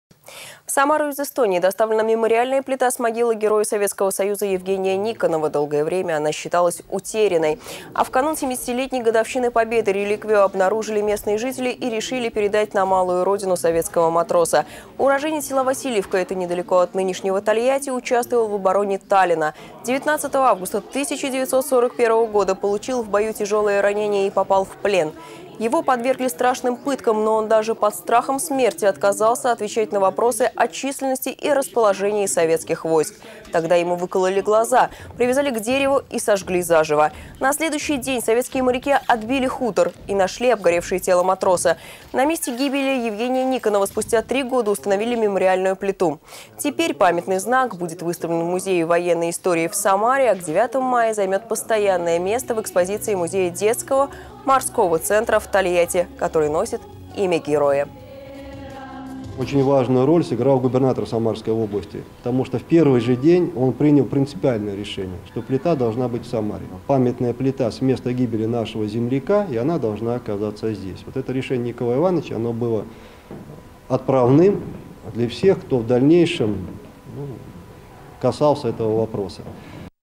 Мемориальная плита с могилы Героя Советского Союза Евгения Никонова отправится в Тольятти
В Самару из Эстонии доставлена мемориальная плита с могилы Героя Советского Союза Евгения Никонова. Долгое время она считалась утерянной, а в канун 70-летней годовщины Победы реликвию обнаружили местные жители и решили передать на малую родину советского матроса. Уроженец села Васильевки - это недалеко от нынешнего Тольятти - участвовал в обороне Таллина. 19 августа 1941-го получил в бою тяжелое ранение и попал в плен. Его подвергли страшным пыткам, но он даже под страхом смерти отказывался отвечать на вопросы о численности и расположении советских войск. Тогда ему выкололи глаза, привязали к дереву и сожгли заживо. На следующий день советские моряки отбили хутор. И нашли обгоревшее тело матроса. На месте гибели Евгения Никонова спустя 3 года установили мемориальную плиту. Теперь памятный знак будет выставлен в Музее военной истории в Самаре, а к 9 мая займет постоянное место в экспозиции музея "Детского морского центра" в Тольятти, который носит имя героя.